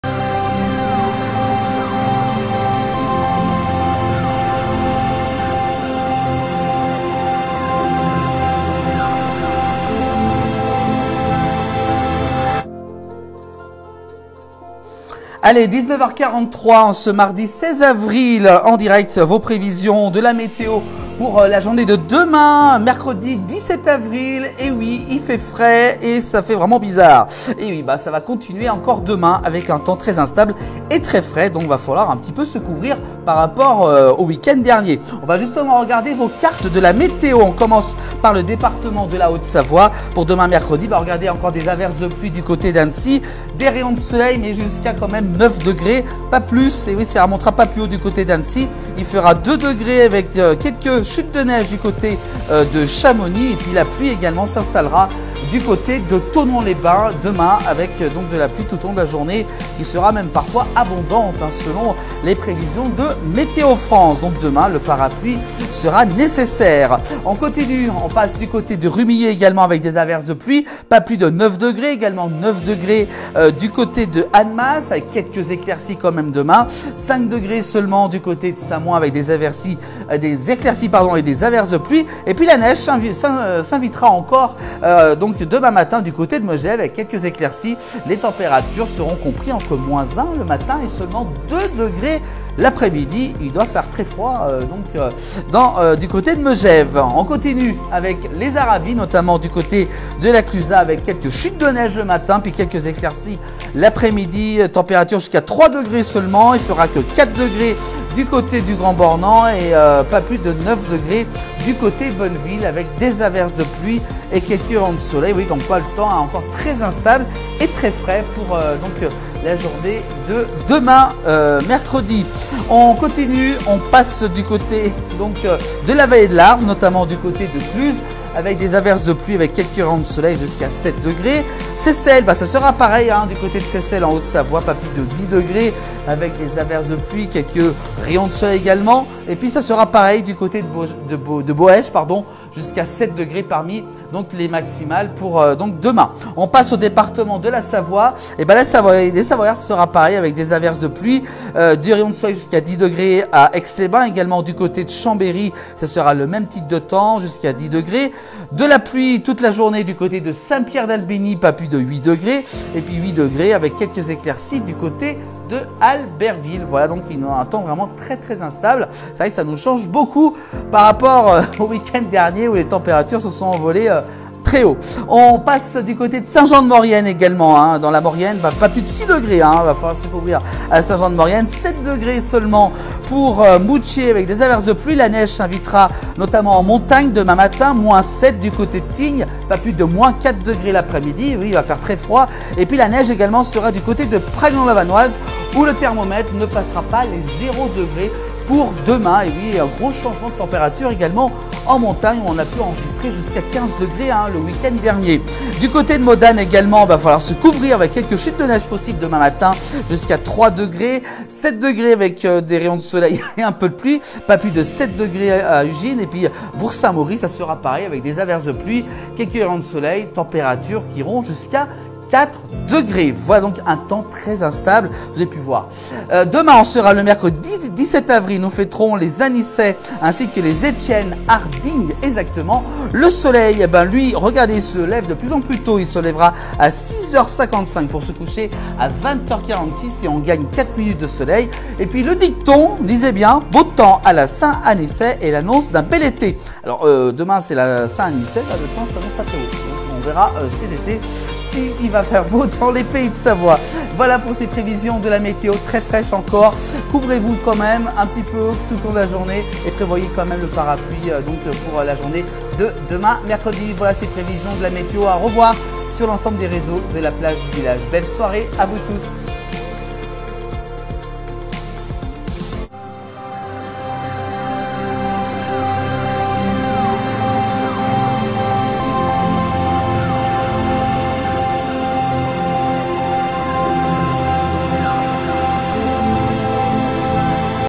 Les prévisions de la météo pour mercredi 17 avril 2024 en Haute-Savoie et Savoie
Les prévisions de la météo pour mercredi 17 avril 2024 en Haute-Savoie et Savoie avec @yohanannecy